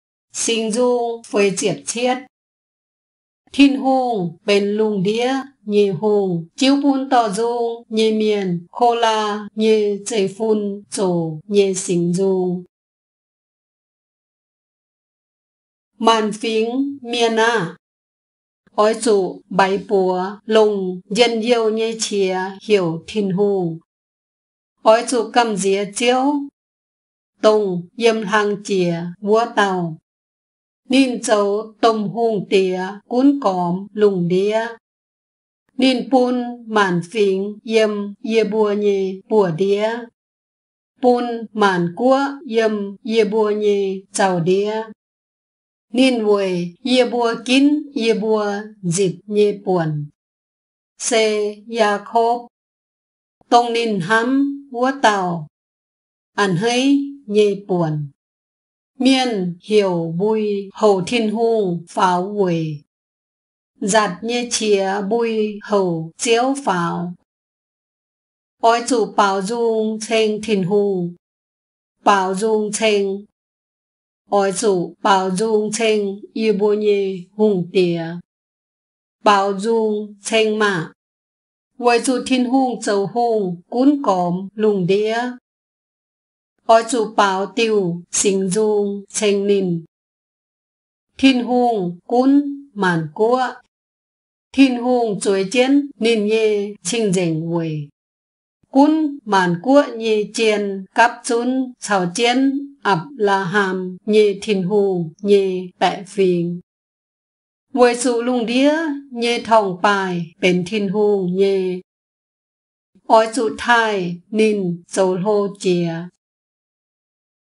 Singx Nzung 47 (Psalm 47) Mien Bible - พระคัมภีร์ภาษาเย้า: สดุดี บทที่ 47
Mien Language Bible: Psalm 47
พระคัมภีร์ภาษาเมี่ยน: สดุดี 47
Mienh nyei Ging-Sou: Singx Nzung 47
เมี่ยน เญย กีง-โซว: สีง ฑูง 47
ມ່ຽນ ເຍີຍ ກີງ-ໂຊວ: ສີງ ດສູງ 47

1 Maanc fingx mienh aah! Oix zuqc mbaix buoz
longc njien-youh nyei qiex heuc Tin-Hungh.
2 Oix zuqc gamh nziex Ziouv, dongh Yiem Hlang Jiex Wuov Dauh.
Ninh zoux domh Hungh Diex gunv gormx lungh ndiev.
3 Ninh bun maanc fingx yiem yie mbuo nyei buoz-ndiev.
Bun maanc guoqv yiem yie mbuo nyei zaux-ndiev.
4 Ninh weic yie mbuo ginv yie mbuo nzipc nyei buonc. *Se Kaa^naa^an Deic-Bung.
Se Yaakopv, dongh ninh hnamv wuov dauh, a'hneiv nyei buonc. Selaa
5 Mienh heuc mbui houc Tin-Hungh faaux weic.
Nzatc nyei qiex mbui houc Ziouv faaux.
6 Oix zuqc baaux nzung ceng Tin-Hungh, baaux nzung ceng.
Oix zuqc baaux nzung ceng yie mbuo nyei Hungh Diex, baaux nzung ceng maah!
7 Weic zuqc Tin-Hungh zoux Hungh gunv gormx lungh ndiev,
oix zuqc baaux diuh singx nzung * ceng ninh.
8 Tin-Hungh gunv maanc guoqv.
Tin-Hungh zueiz jienv ninh nyei cing-nzengc weic.
9 Gunv maanc guoqv nyei jien gapv zunv
caux jienv Apc^laa^ham nyei Tin-Hungh nyei baeqc fingx.
Weic zuqc lungh ndiev nyei torngv-baaih * benx Tin-Hungh nyei.
Oix zuqc taaih ninh zoux hlo jiex.

1 หม่าน ฝีง เมี่ยน อ้า! อ๋อย หฒุ ไบ๋ ปั์ว
หล่ง เฌียน-โย่ว เญย เฉีย เห่ว ทิน-ฮู่ง.
2 อ๋อย หฒุ กั้ม เหฑีย เฒี้ยว, ต้ง เยียม ฮลัง เจี๋ย วั้ว เต้า.
นิ่น โหฒว ต้ม ฮู่ง เตี๋ย กุ๊น ก๋อม ลู่ง เดี๊ย.
3 นิ่น ปุน หม่าน ฝีง เยียม เยีย บัว เญย ปั์ว-เดี๊ย.
ปุน หม่าน กั๊วะ เยียม เยีย บัว เญย เหฒา-เดี๊ย.
4 นิ่น เหว่ย เยีย บัว กิ๊น เยีย บัว หฑิบ เญย ป่วน. *
เซ ยา^โค้บ, ต้ง นิ่น ฮนั้ม วั้ว เต้า, อะเฮน้ย เญย ป่วน. เซ^ลา
5 เมี่ยน เห่ว บุย โห่ว ทิน-ฮู่ง ฝาว เหว่ย.
หฑัด เญย เฉีย บุย โห่ว เฒี้ยว ฝาว.
6 อ๋อย หฒุ ป๋าว ฑูง เธง ทิน-ฮู่ง, ป๋าว ฑูง เธง.
อ๋อย หฒุ ป๋าว ฑูง เธง เยีย บัว เญย ฮู่ง เตี๋ย, ป๋าว ฑูง เธง ม่าะ!
7 เหว่ย หฒุ ทิน-ฮู่ง โหฒว ฮู่ง กุ๊น ก๋อม ลู่ง เดี๊ย,
อ๋อย หฒุ ป๋าว ติ้ว สีง ฑูง * เธง นิ่น.
8 ทิน-ฮู่ง กุ๊น หม่าน กั๊วะ.
ทิน-ฮู่ง เฒว์ย เจี๊ยน นิ่น เญย ธีง-เหฑ่ง เหว่ย.
9 กุ๊น หม่าน กั๊วะ เญย เจียน กั๊บ ฒุ้น
เหธา เจี๊ยน อับ^ลา^ฮัม เญย ทิน-ฮู่ง เญย แปะ ฝีง.
เหว่ย หฒุ ลู่ง เดี๊ย เญย ท้อง-ป้าย * เป๋น ทิน-ฮู่ง เญย.
อ๋อย หฒุ ท่าย นิ่น โหฒว โฮล เจี๋ย.